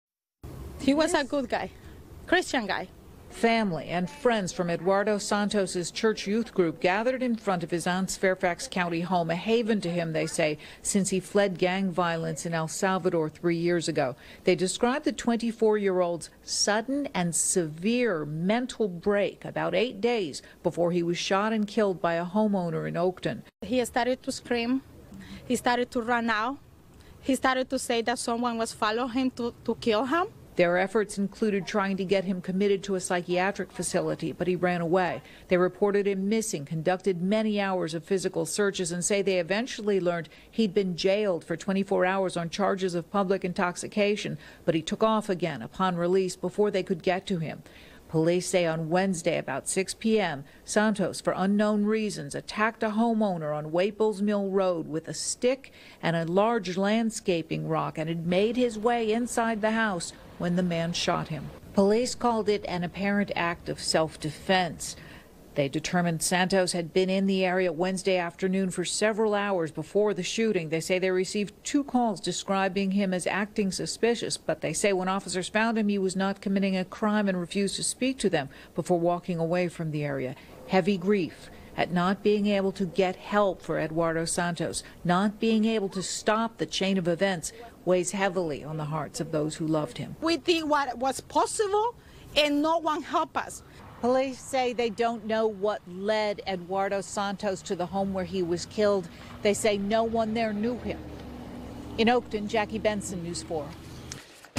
Friends, Family Say Man Killed in Virginia Home Suffered Mental Break | NBC4 Washington
A man shot to death in a Virginia home recently had a mental break and needed help, according to family and friends. News4’s Jackie Bensen reports.